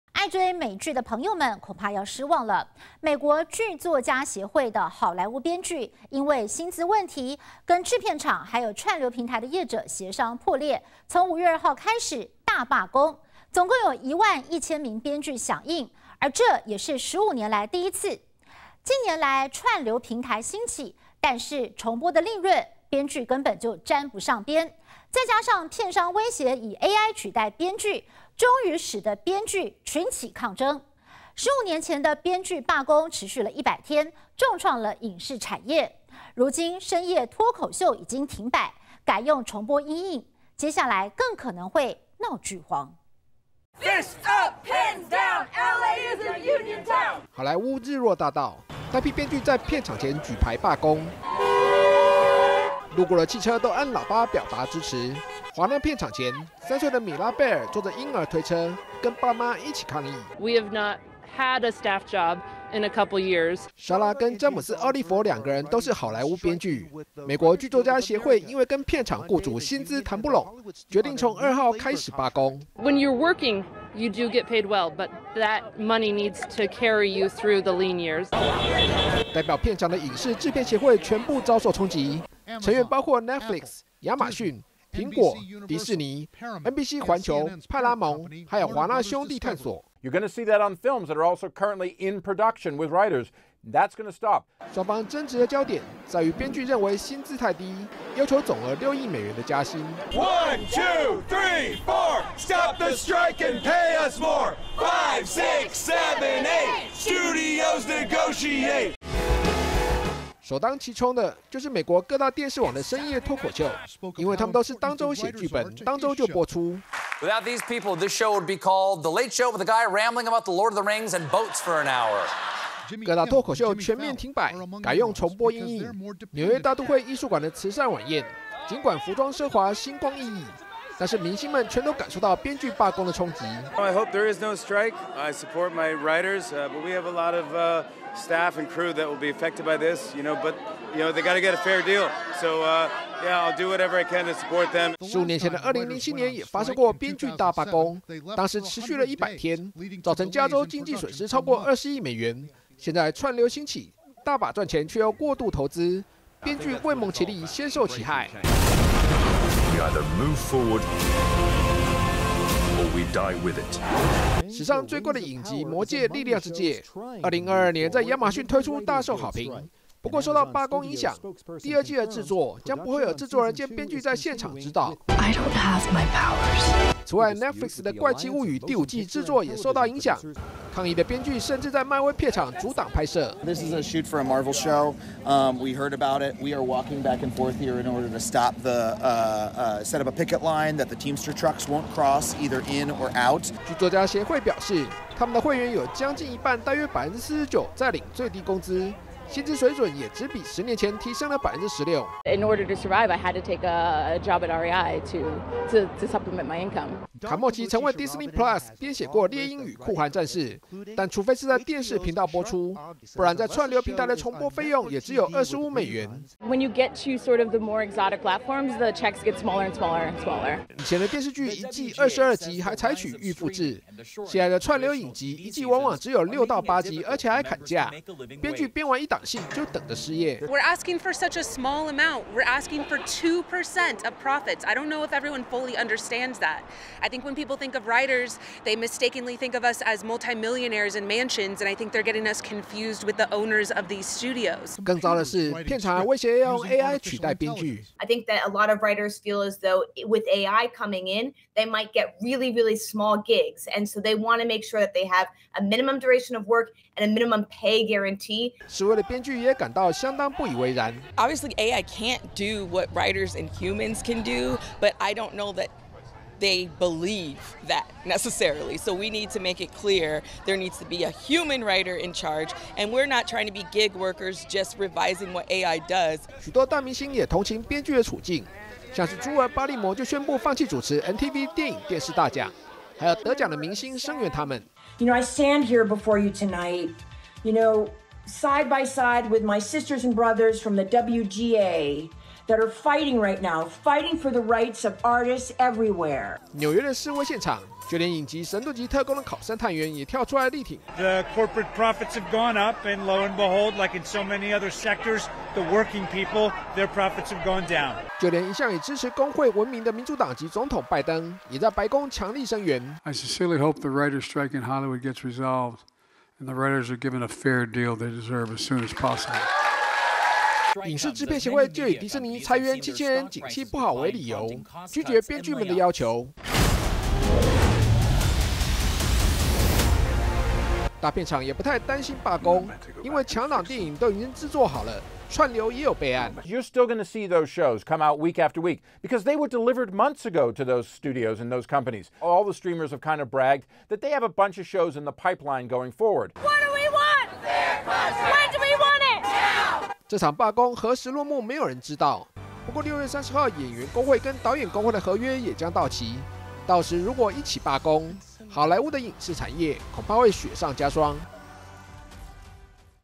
益起看世界-美國好萊塢編劇大罷工 因薪酬問題與片商協商破裂 串流平台興起 重播報酬無關編劇 片商威脅AI取代編劇 深夜脫口秀停擺重播因應 未來恐鬧劇荒
愛追美劇的朋友們恐怕要失望了。
美國劇作家協會的好萊塢編劇因為薪酬問題，跟片廠還有串流業者協商破裂，從5月2號起展開15年來首次的大罷工，總共有1萬1千名編劇響應。
近年來因為串流興起，但是編劇都拿不到重播的報酬，加上片商威脅用AI取代編劇，都引發編劇的強烈不滿。
15年前的罷工持續一百天，重創了影視產業，不過這次片廠卻說他們自己都在裁員，雙方歧見佷深，深夜脫口秀已經正式停擺改用重播因應，接下來很可能就會鬧劇荒。